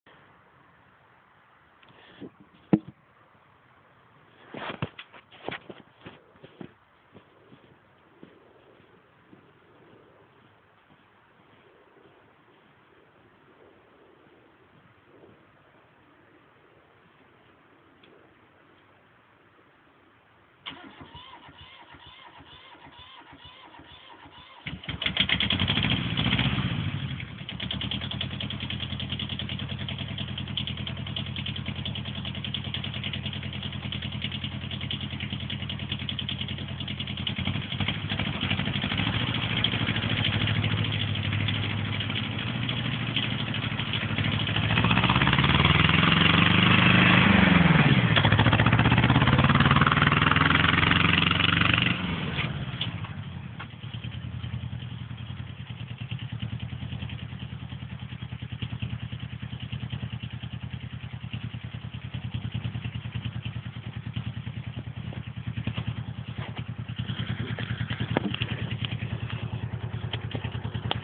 Kubota B-6000 mit Anhänger
Beschleunigung mit voll beladenem Multicaranhänger